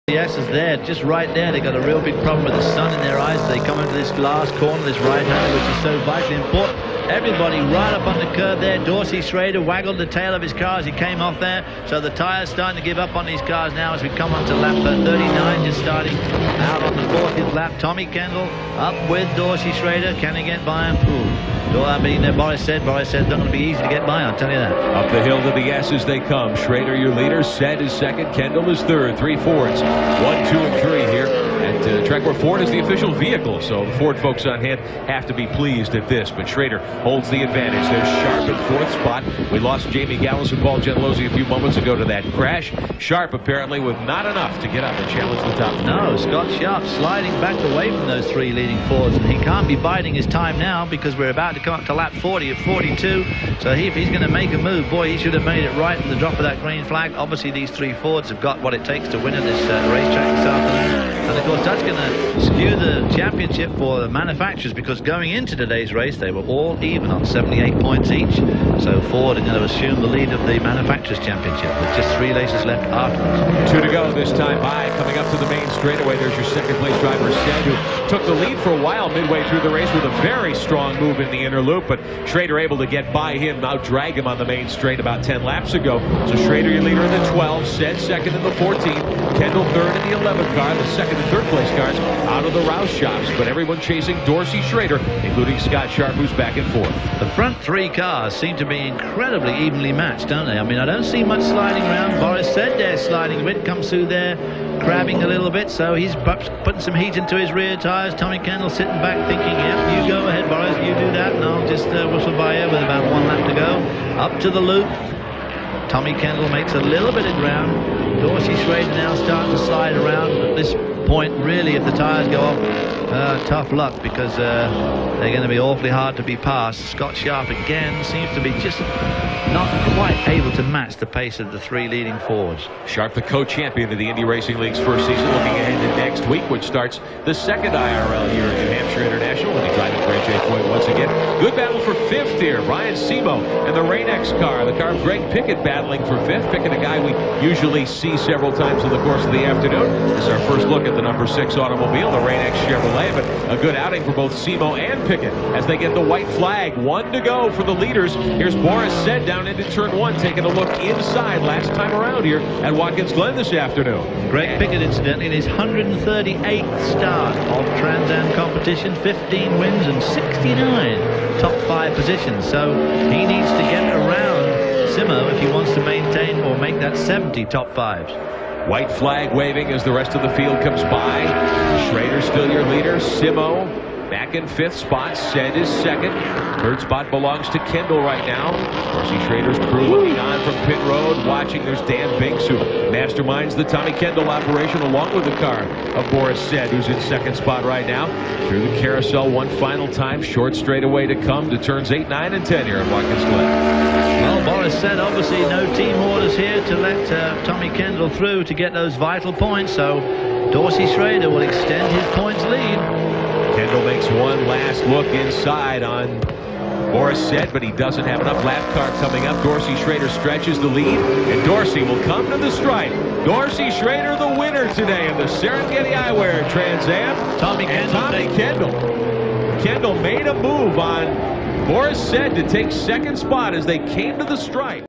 Schroeder Outdrives Kendall and Said for the Win (1996 Watkins Glen Flashback)
Dorsey Schroeder held on to take the checkered flag at Watkins Glen in 1996, holding off Trans Am greats like Tommy Kendall, Boris Said and even Scott Sharp.

Trans Am returns to upstate New York for the Watkins Glen SpeedTour event Sept. 10-12.